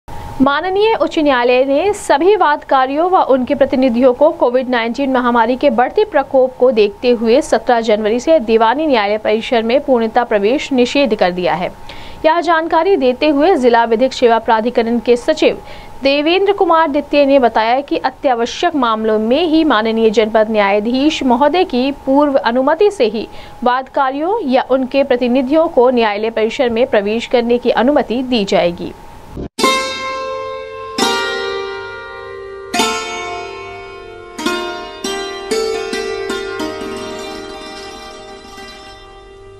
कोविड-19 महामारी के बढ़ते प्रकोप को देखते हुए 17 जनवरी से दीवानी न्यायालय परिसर में वादकारियों या
कोविड-19 महामारी के बढ़ते प्रकोप को देखते हुए 17 जनवरी से दीवानी न्यायालय परिसर में वादकारियों या उनके प्रतिनिधियों को उच्च न्यायालय ने किया पूर्णतः प्रतिबंधित, अत्यावश्यक मामलों में जनपद न्यायाधीश की पूर्व अनुमति से ही वादकारियों या उनके प्रतिनिधियों को न्यायालय परिसर में प्रवेश करने की दी जाएगी अनुमति